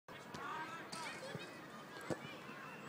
PVCV E11 - DWSV E2 (10-12-2011)
kampioen
Een fragment uit de wedstrijd PVCV E11 tegen DWSV E2 op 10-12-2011.
De wedstrijd werd gespeeld op Fletiomare Oost sportp. - PVCV te VLEUTEN en eindigde in een 1 - 3 winst voor DWSV E2.